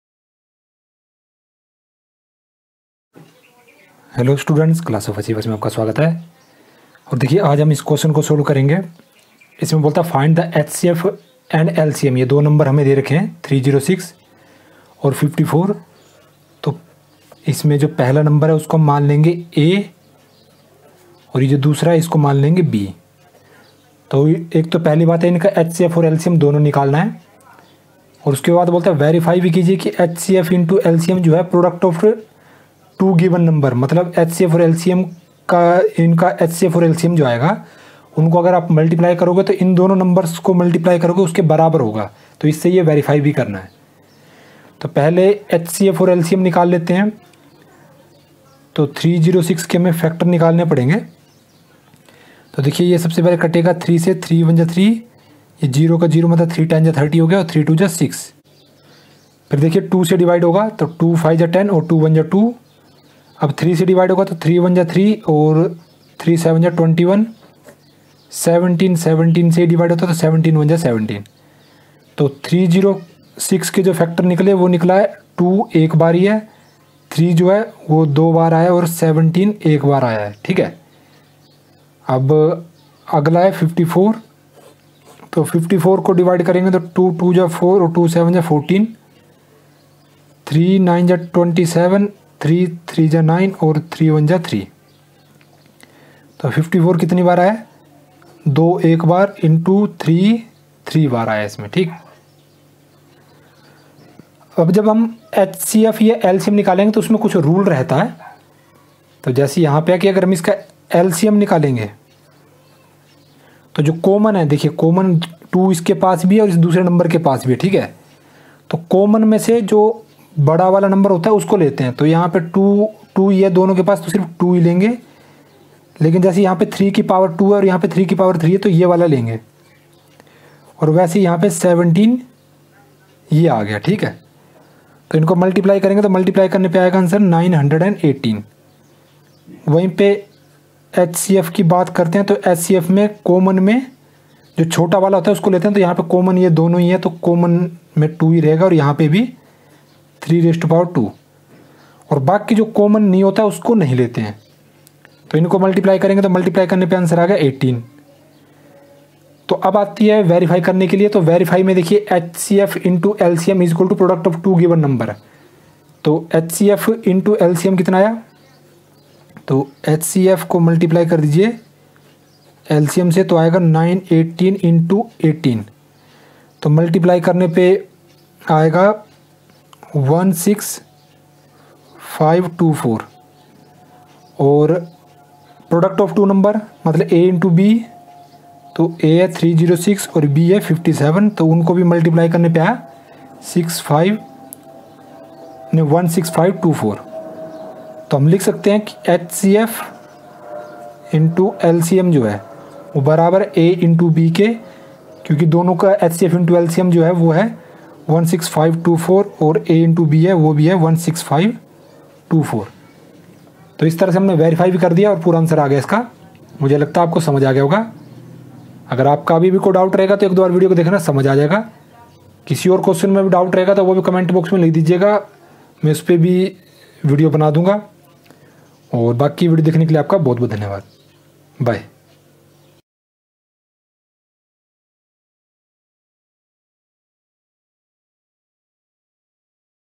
Find the HCF and LCM of 306 and 54. Also, verify that HCF X LCM = Product of given two numbers
Find the HCF and LCM of 306 and 54. Also, verify that HCF X LCM = Product of given two numbers. Class 10